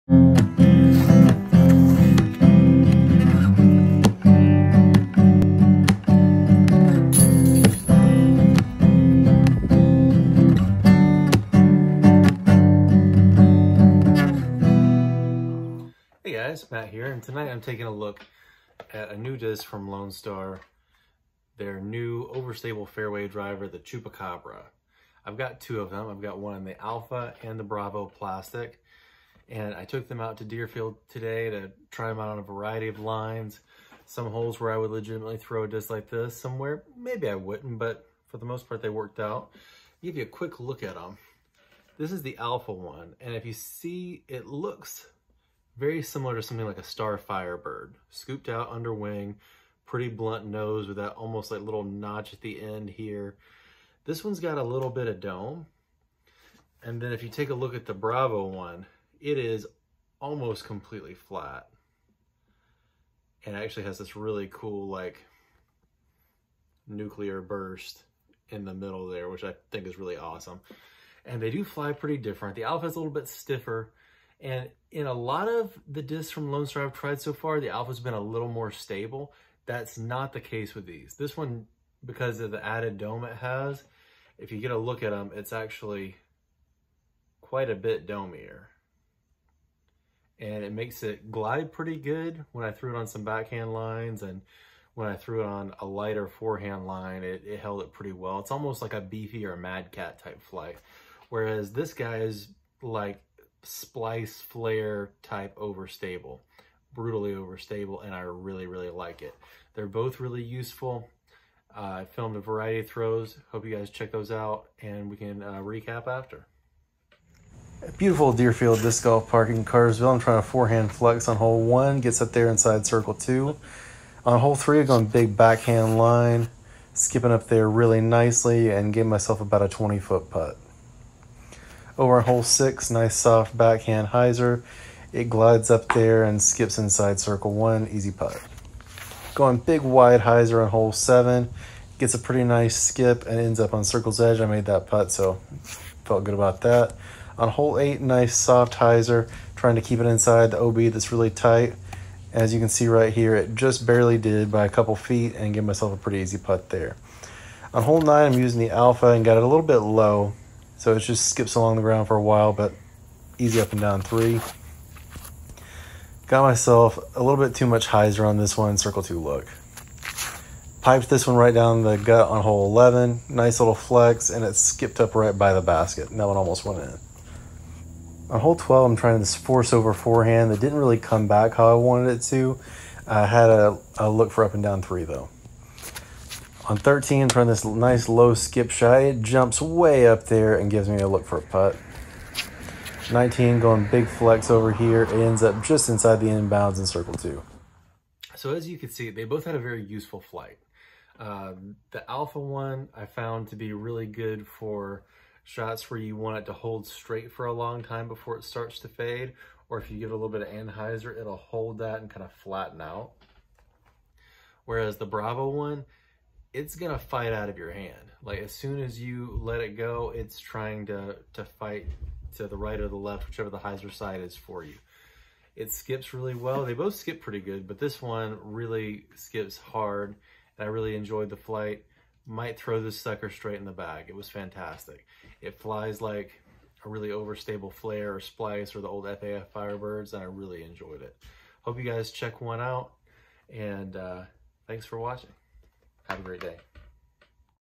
Lonestar Chupacabra, taking a bite out of the course
A quick review of the new Lonestar Disc Chupacabra in both Alpha and Bravo plastic at the Deerfield Disc Golf Park.